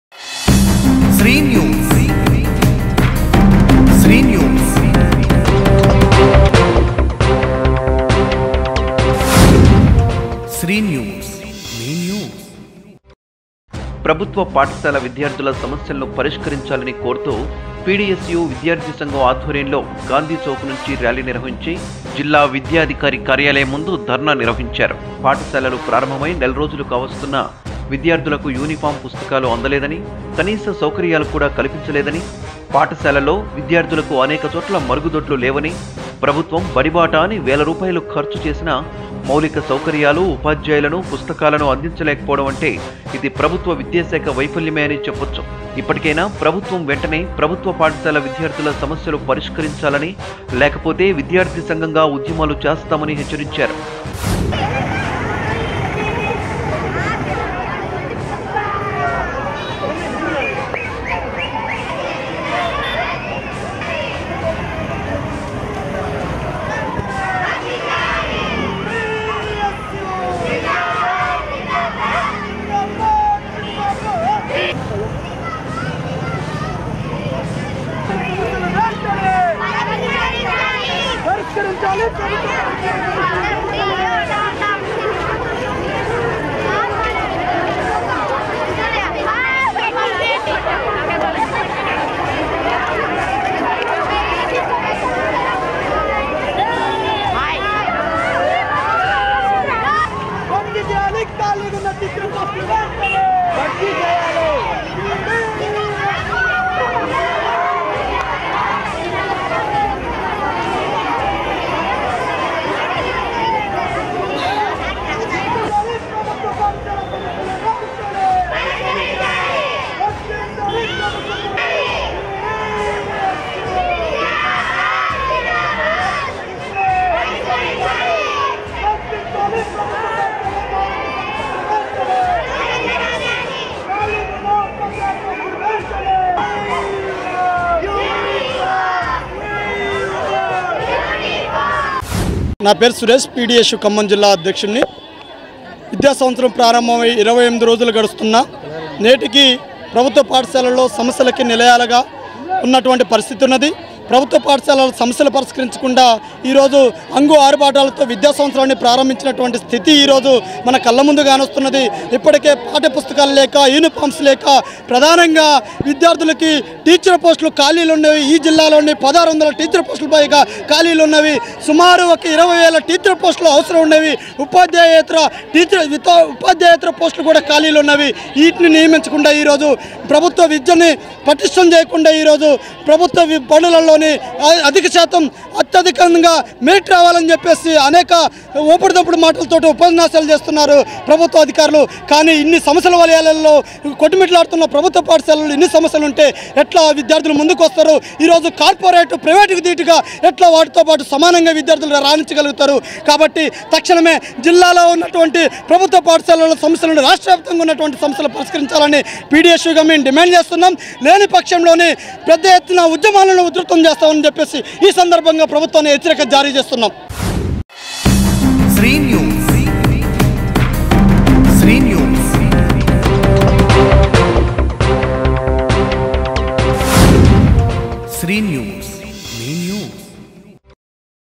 ఖమ్మంలో PDSU ఆధ్వర్యంలో ర్యాలీ, ధర్నా...ప్రభుత్వ పాఠశాల విద్యార్ధుల సమస్యలు పరిష్కరించాలని డిమాండ్
sreenews khammam
khammam latest local news updates